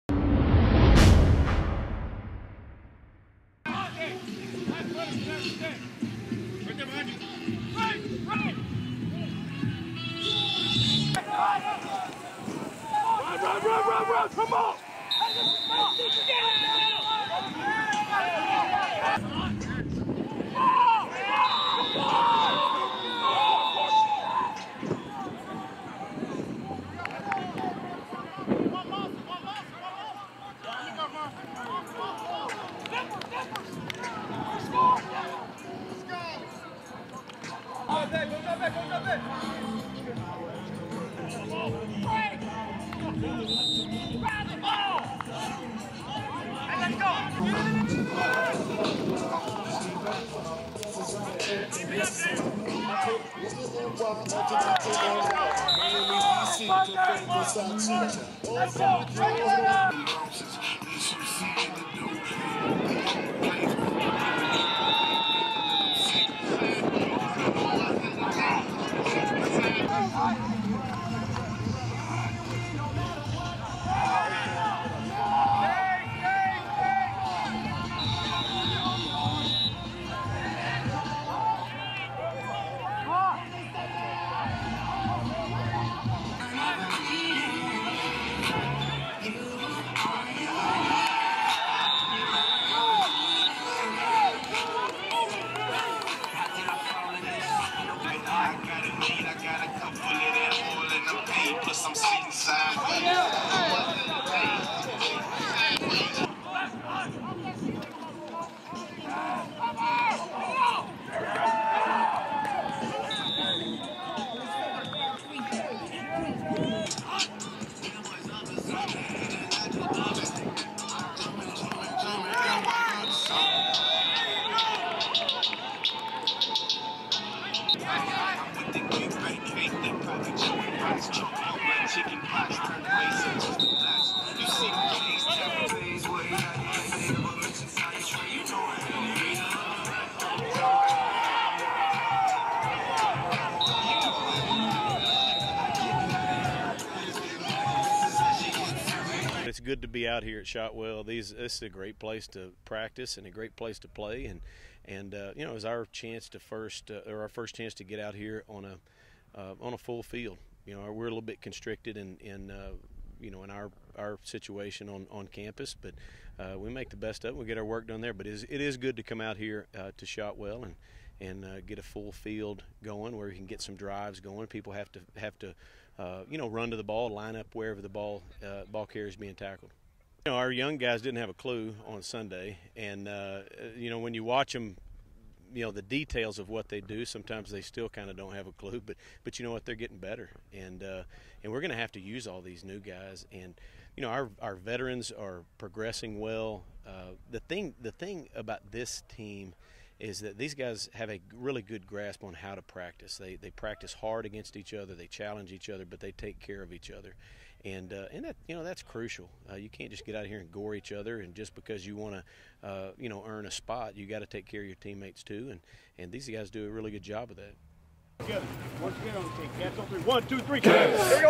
ACU Football | Aug 13 Practice
Video highlights from the Wildcats afternoon practice at Shotwell Stadium, including head coach Ken Collums giving his thoughts on the first week of fall camp.